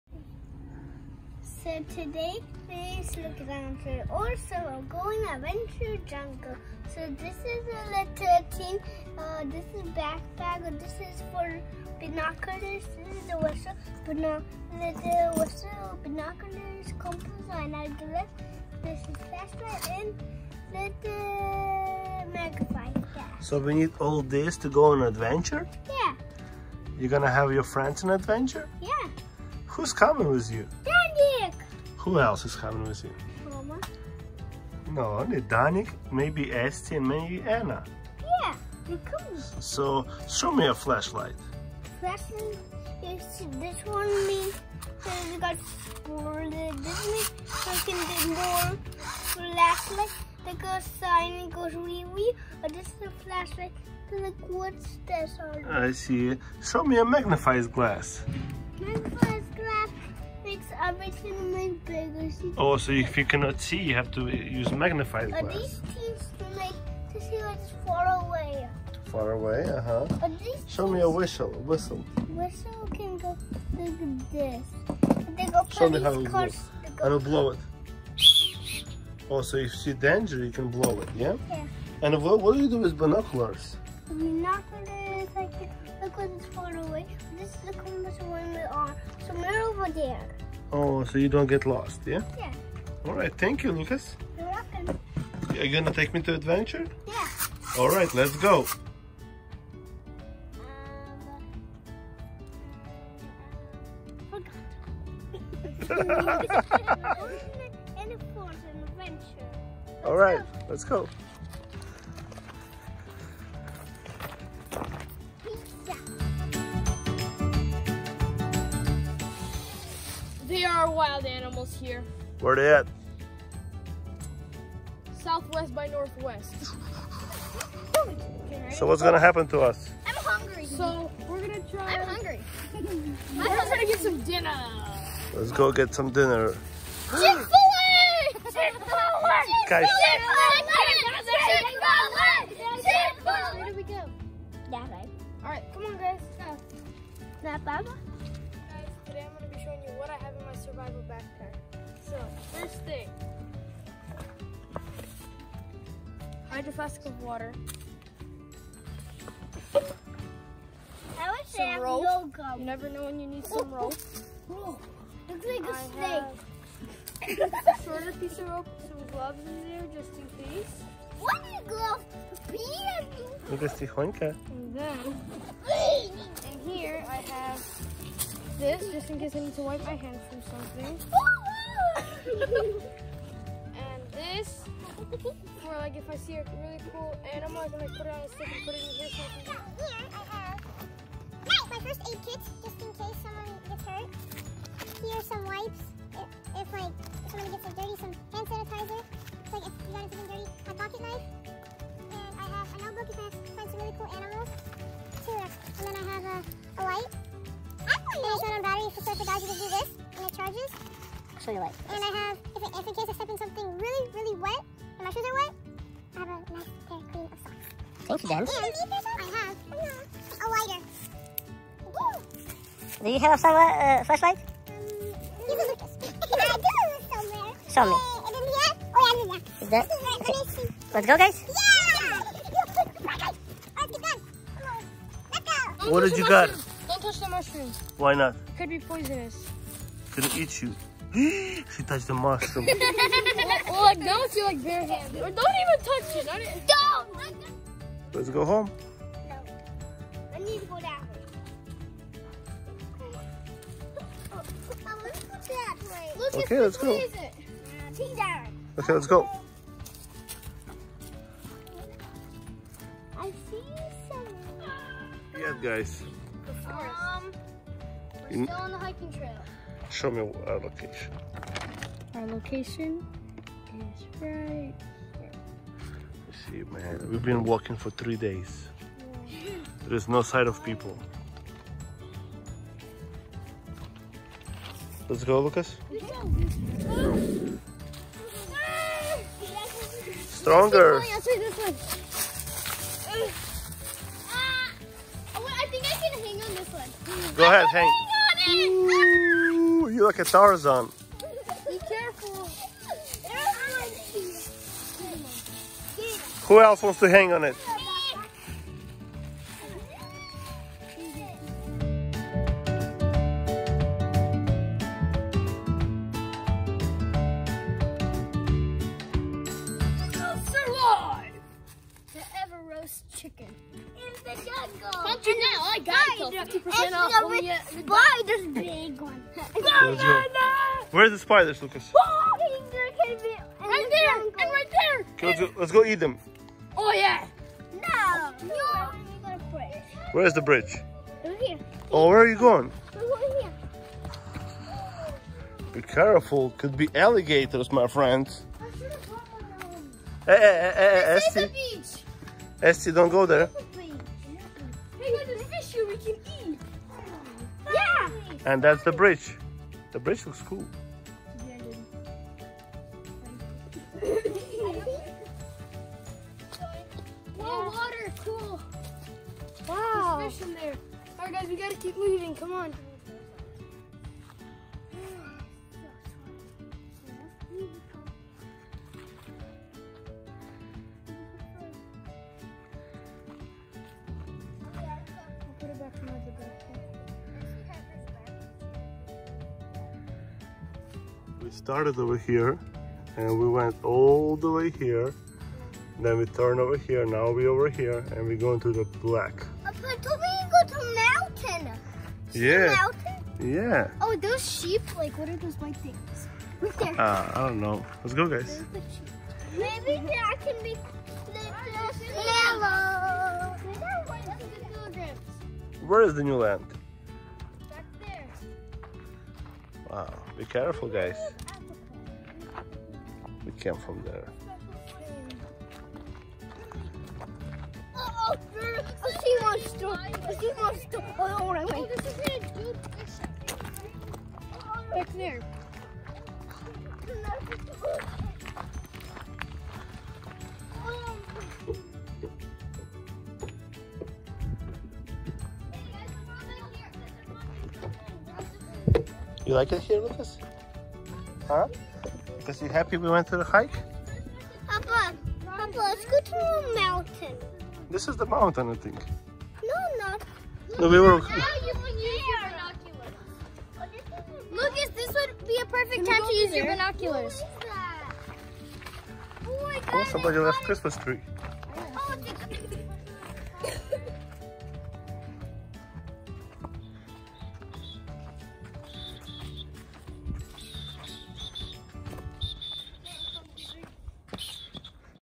Lucas Adventure Kid Vlog 001
Lucas and his friends going on small hike in Jacksonville Florida. The park is Timucan Preserve parking Trails. enjoy